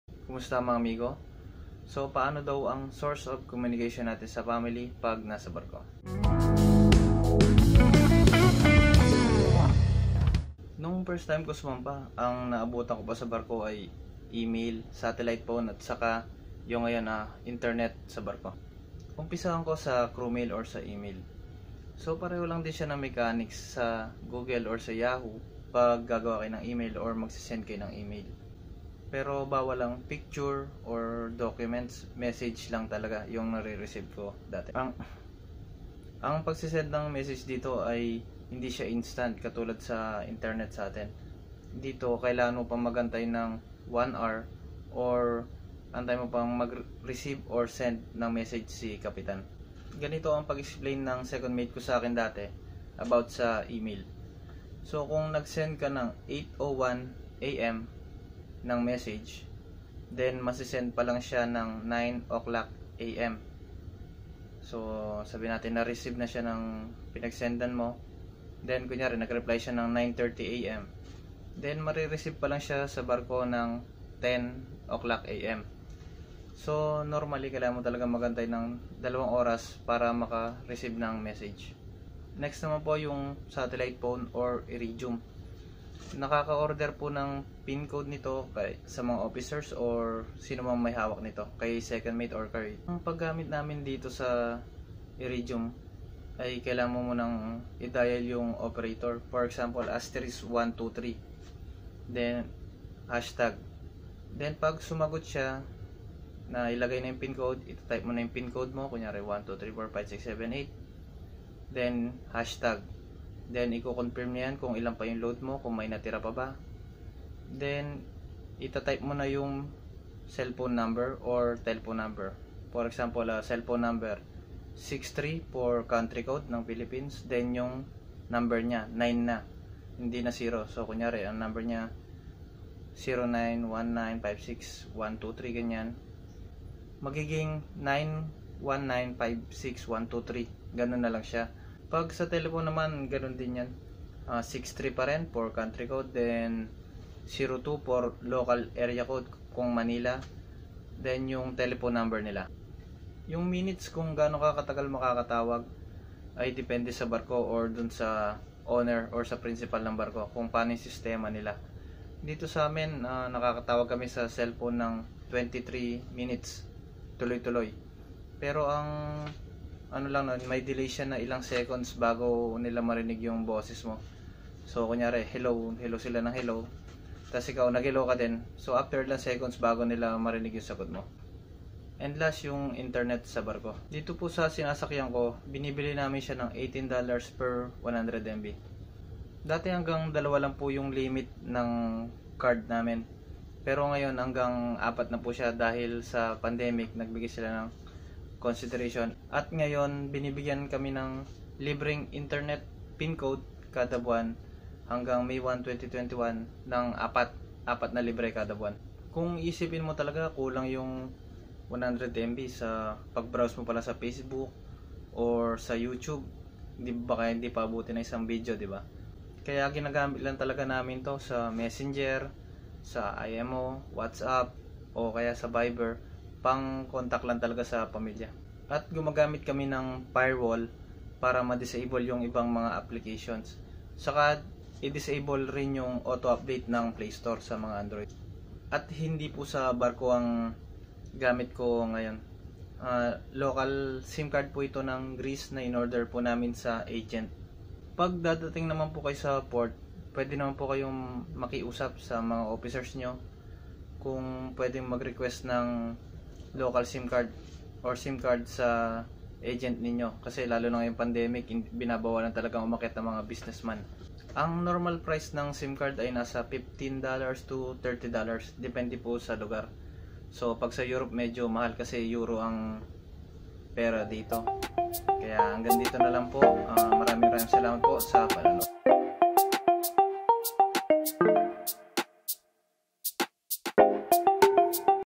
Seaman Vlog ep 23 - what are the Communications onboard the vessel
1 sub = helping me to inform more aspiring seafarer

What are the types of communications onboard the vessel while in the middle of the sea..